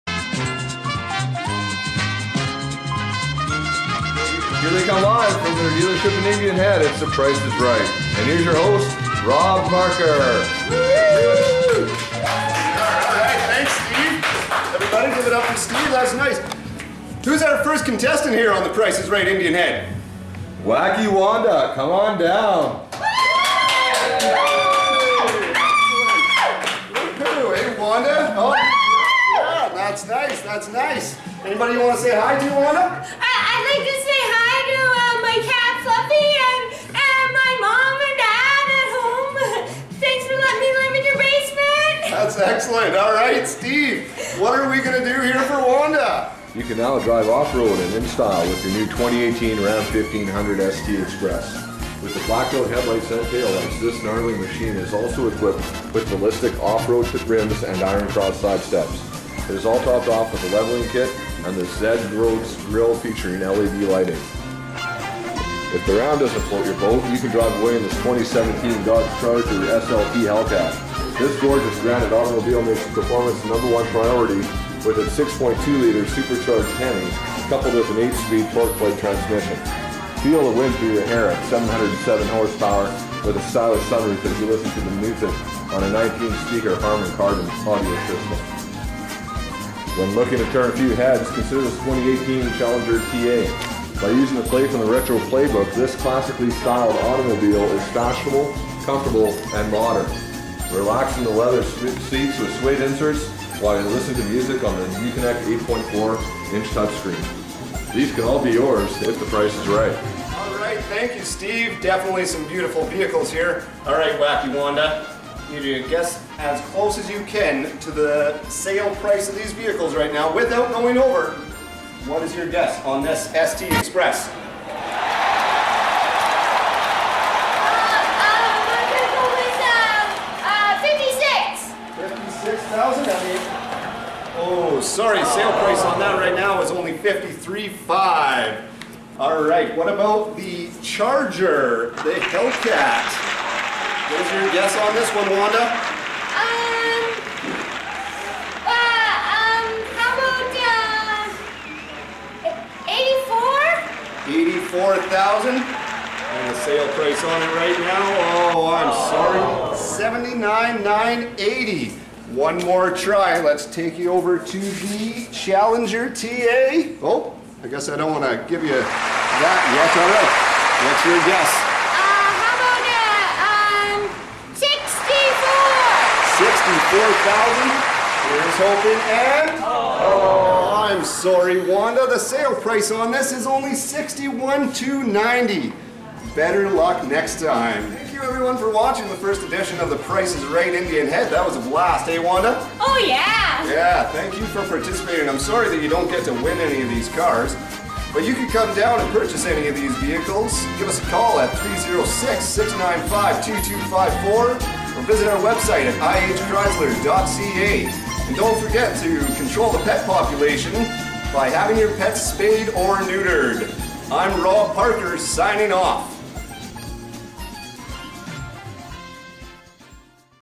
Price is right
A fun look at three of the great vehicles here at Indian Head Chrysler